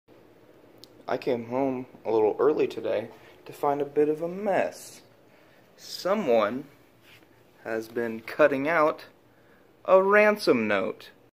Dog has made a BIG mess
I came home to a serious mess my dog Georgia made. Everything has been sorted out now.